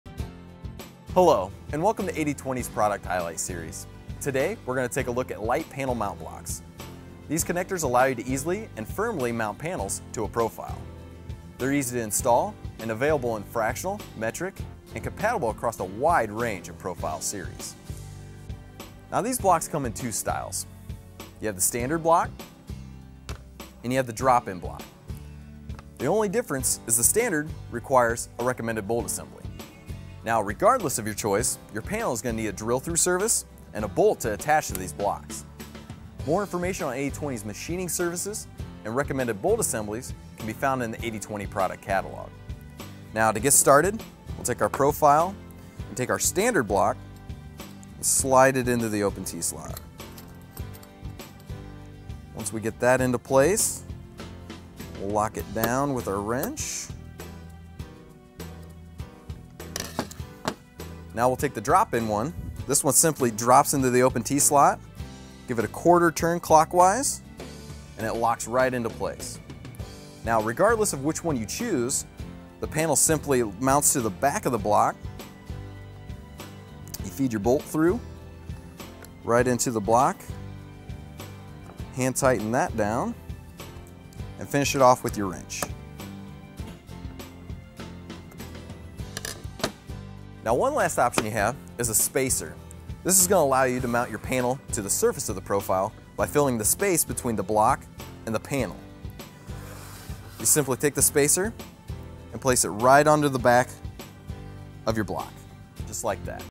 80/20: Lite Panel Mount Block Overview
Hello, and welcome to 80/20's Product Highlight Series. Today we’re going to take a look at Lite Panel Mount Blocks.  These connectors allow you to easily, and firmly mount panels to a profile.

They are easy to install and are available in fractional, metric, and compatible across wide range of profile series.

Now these blocks come in two styles, you have the standard block, and you have the drop in block.  The only difference is that the standard requires a recommended bolt assembly.  Now regardless of your choice, your panels going to need a drill through service, and a bolt to attach to these blocks.

More information on 80/20's machining services and recommended bolt assemblies, can be found in the 80/20 product catalog.

Now to get started, take our profile, take our standard block, and slide it into the open Tslot.  Once we get that into place, we’ll lock it down with our wrench.  Now we’ll take our drop in one.  This one simply drops into the open Tslot, give it a quarter turn clockwise, and it locks right into place.  Now regardless of which on your choose, the panel simply mounts to the back of the block.  You feed you’re bolt through, right into the block, hand-tighten that down, and finish it off with your wrench.

Now one last option you have, is a spacer.  This if going to allow you to mount your panel to the surface of the profile, by filling the space between the block and the panel.  You simply take the spacer and place it right onto the back of the block, just like that.

Now hopefully you found this segment on lite panel mount blocks helpful.
And as always make it a great day!